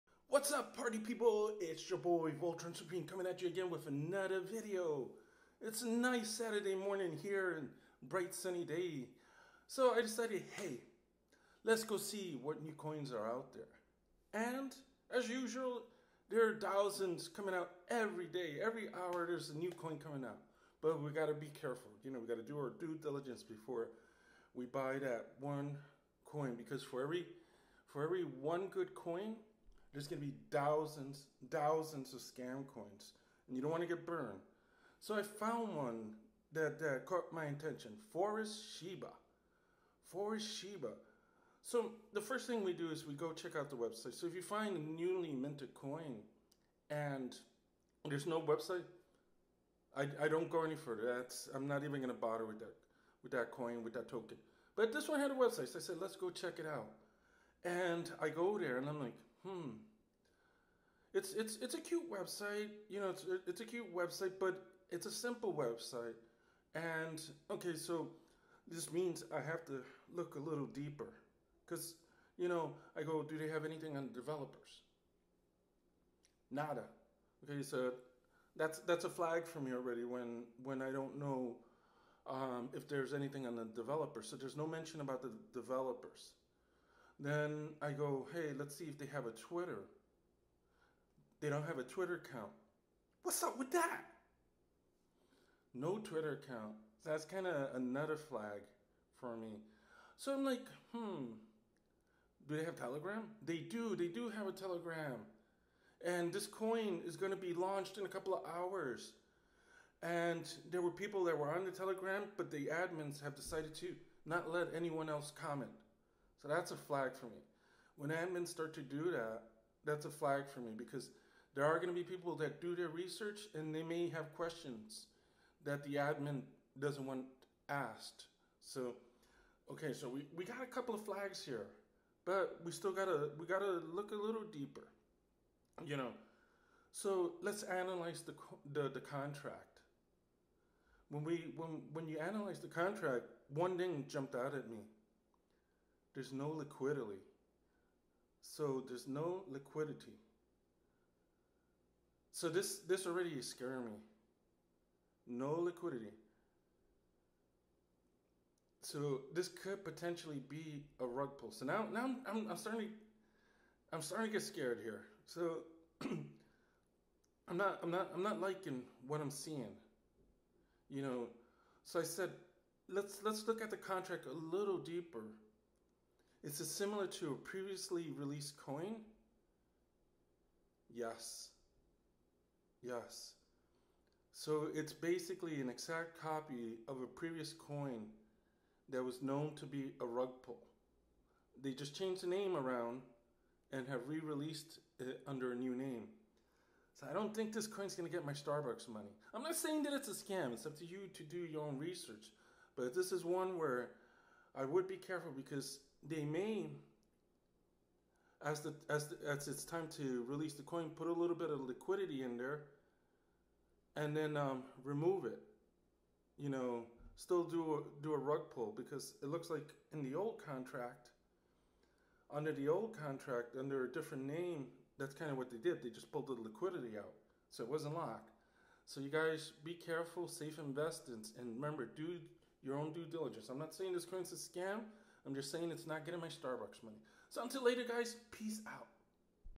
ForestShiba $FORS SCAM?
Today we look at a coin that will be released in a few hours and see whether it is work me putting my starbucks miney into. I am not a financial advisor. Please do your own due diligence.

Forest Shiba